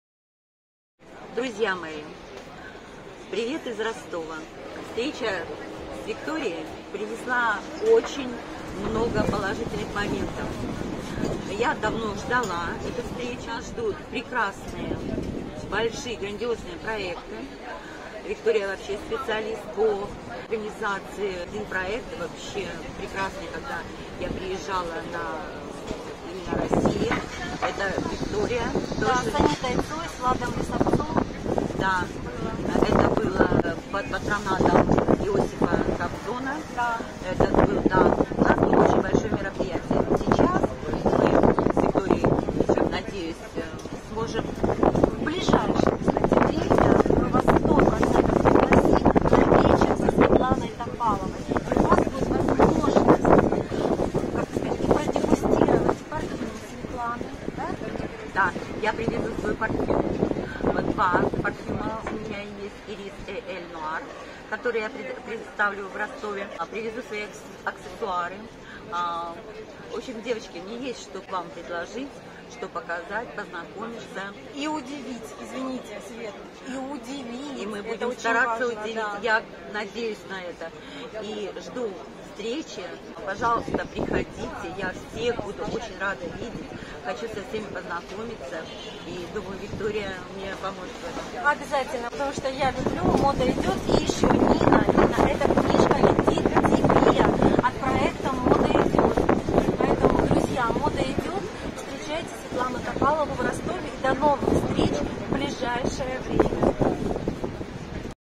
Live диалог с Светланой Топаловой.
Live диалог о совместных творческих планах с моей подругой,организатором fashion мероприятий Викторией Луговской✨